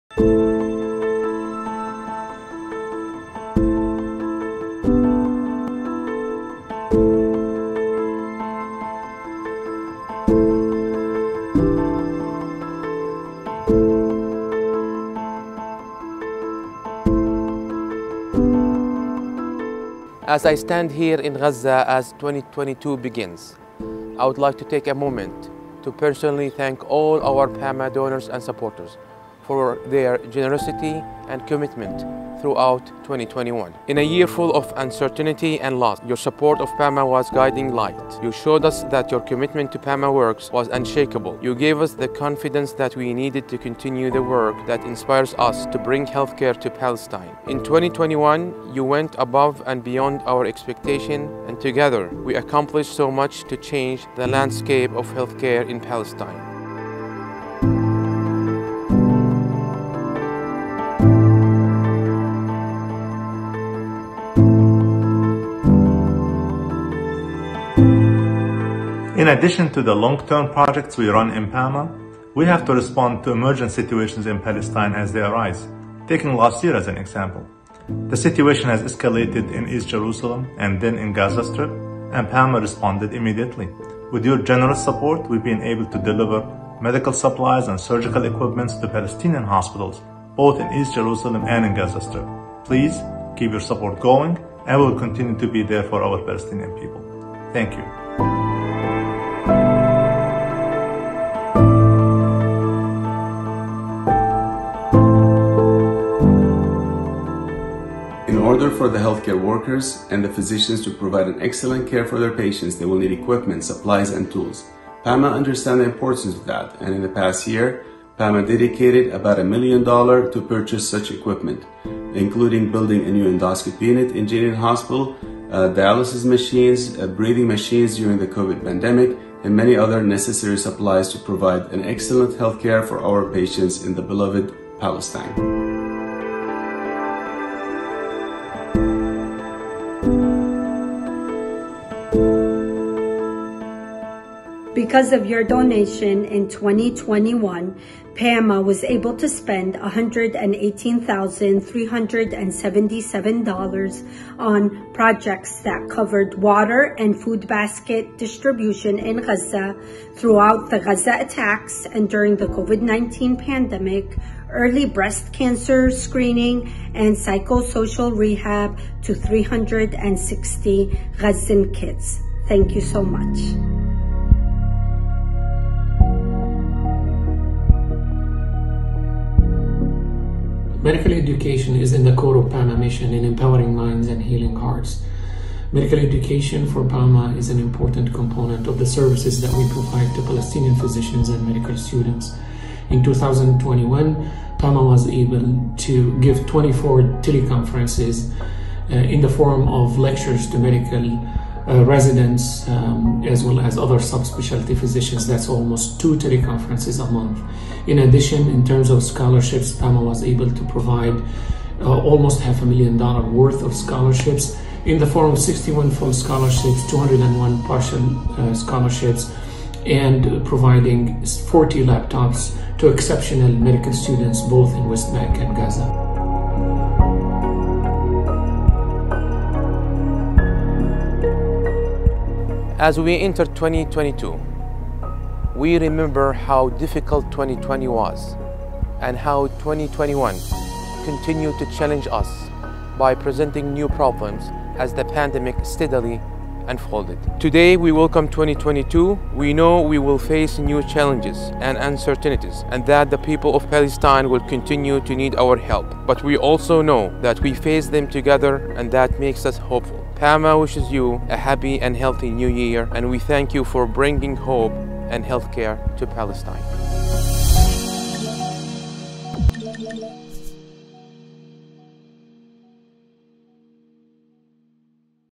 PAMA 2021
2021 was a year full of challenges; but with the generous donations of PAMA's supporters, we were able to help the patients and the healthcare in Palestine.